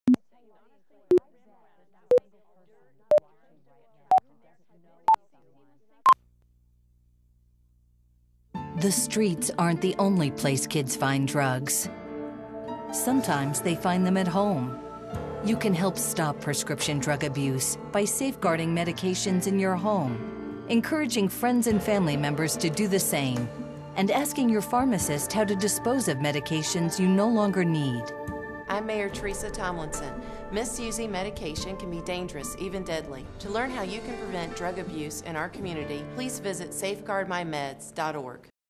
Mayor Teresa Tomlinson: "Safeguard My Meds - 2011 Prescription Drug Abuse Awareness Campaign"
Did you know that according to recent research, every day more than 2,500 teenagers abuse prescription medication for the first time?   In order to address this growing public health probelm, The U.S. Conference of Mayors and The National Community Pharmacists Assoication in conjuction w/Purdue Pharma L.P. have joined together to support a notional awareness initiative call Safegueard My Meds, which aims to help reduce the abuse of prescription medications.